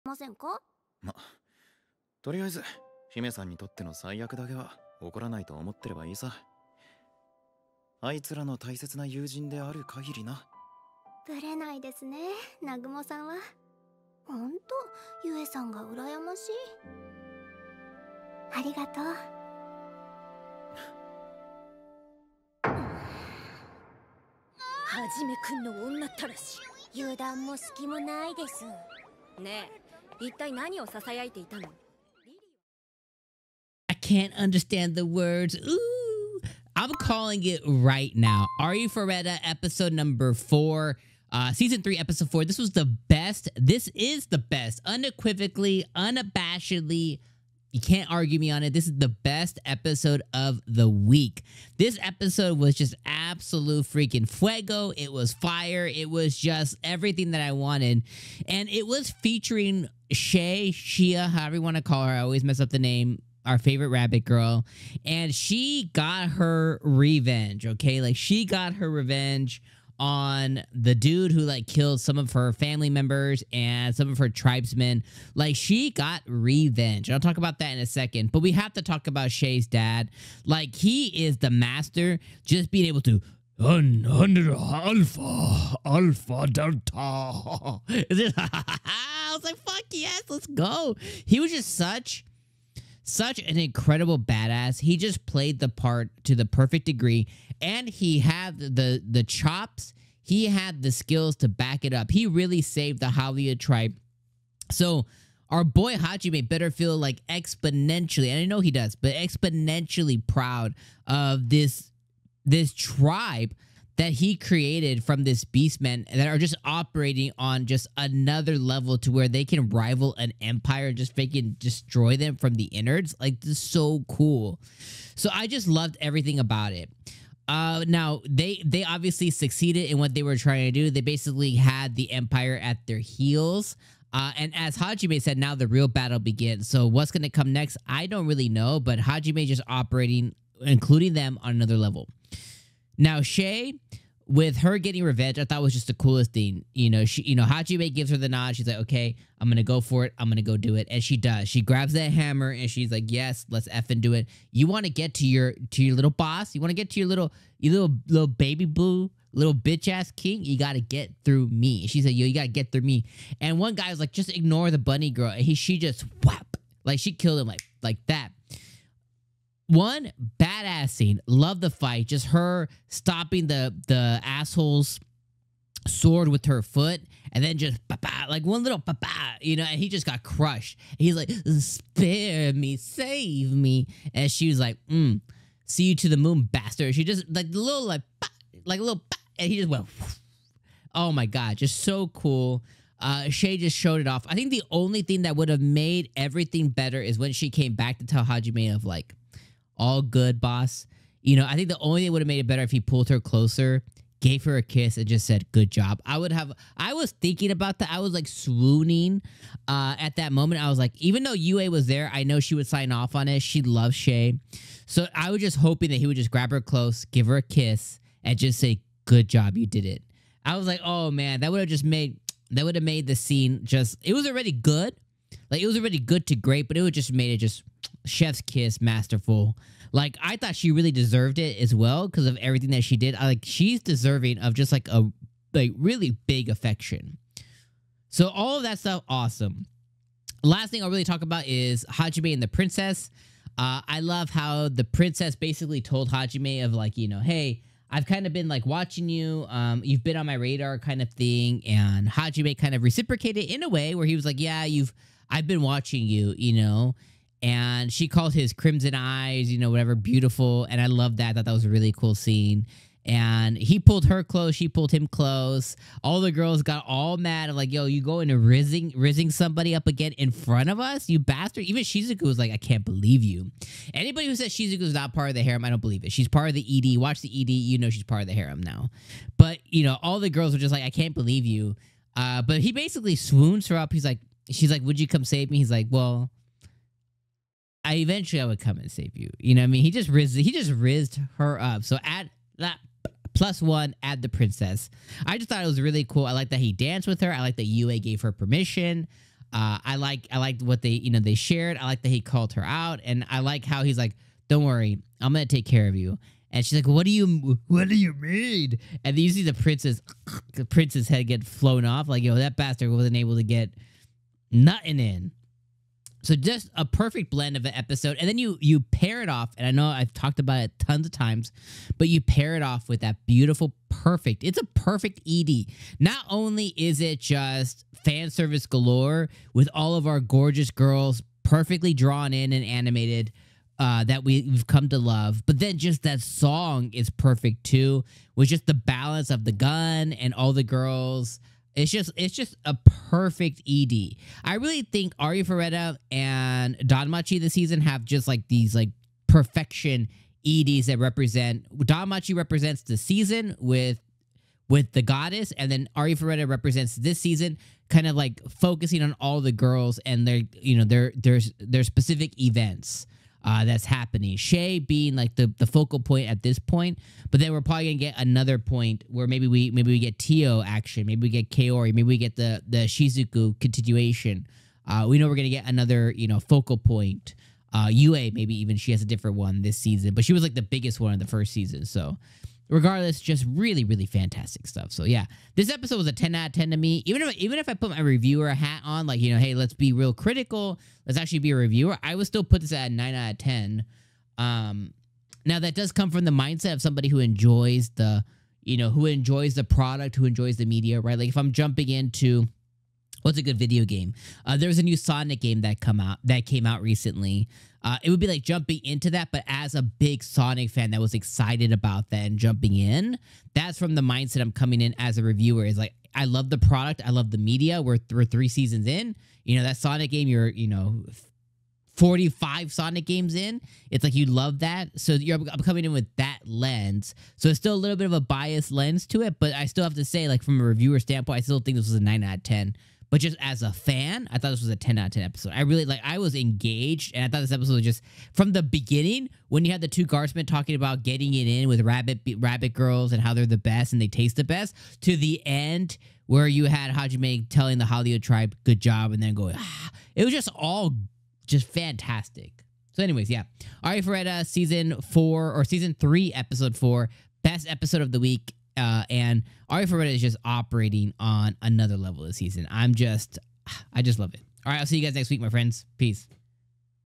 Arifureta S3 Ep 4: Bunny Army Wins, Empire Falls & Hajime’s Rizz Game with the Princess?! 🐰👑
In Arifureta Season 3 Episode 4, Hajime leads a successful attack with the Bunny Army, conquering the empire and sparking romance with the princess! This episode brings intense action, major wins, and fan-favorite moments that set a new high for the season. Catch all the details in our review!